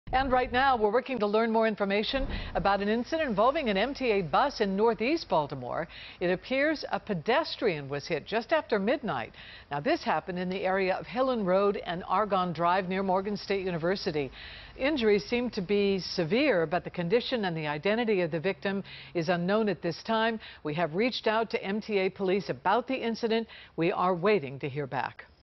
Pedestrian in critical condition following collision with MTA bus
The man was traveling on a scooter near the intersection of Hillen Road and Argonne Drive when the bus struck him a few minutes after midnight.